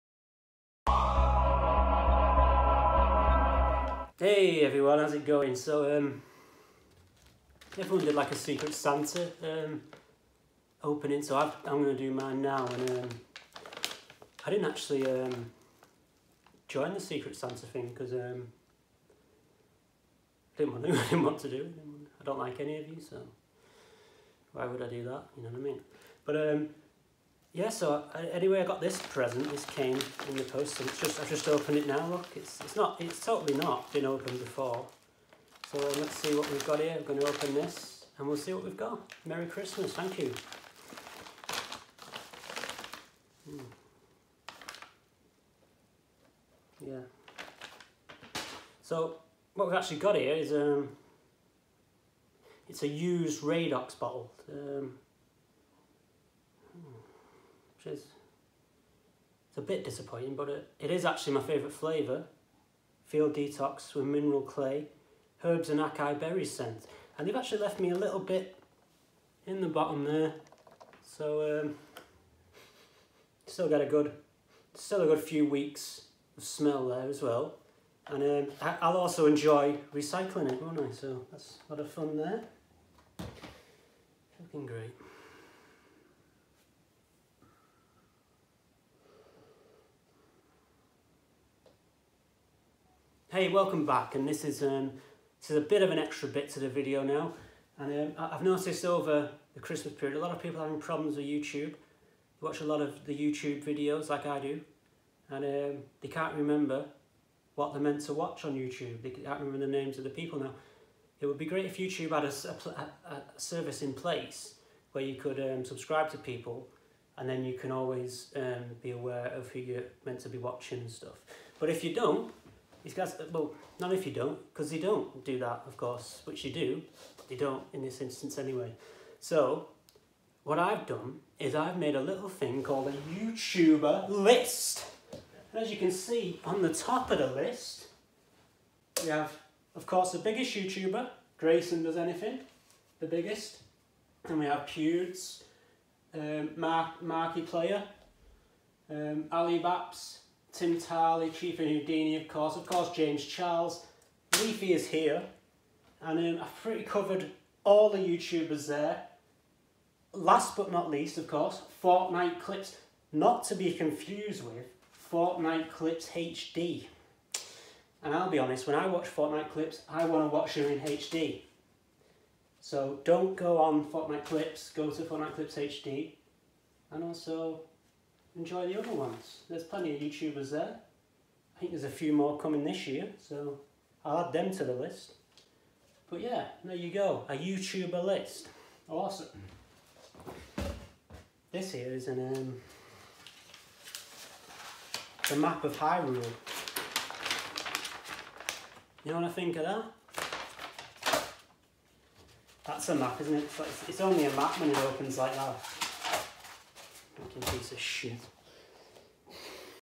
Secret santa and youtuber list
I open my secret santa from the mossy reach crew and share my tips on watching youtube videos